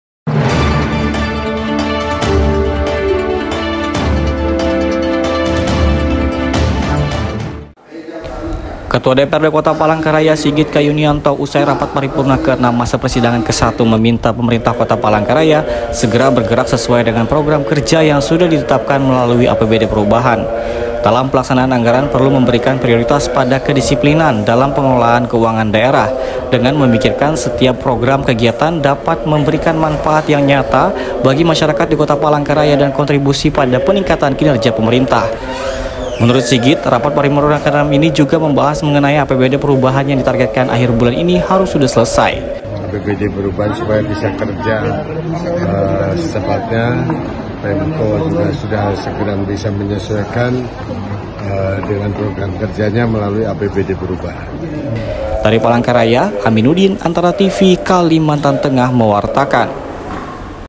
DPRD Palangka Raya minta pemkot gerak cepat lanjutkan pembangunan
ANTARA - Ketua DPRD Kota Palangka Raya, Kalimantan Tengah, Sigit K Yunianto meminta Pemerintah Kota setempat segera bekerja sesuai dengan apa yang sudah direncanakan, untuk melanjutkan pembangunan di daerah tersebut. (Amin Nudin)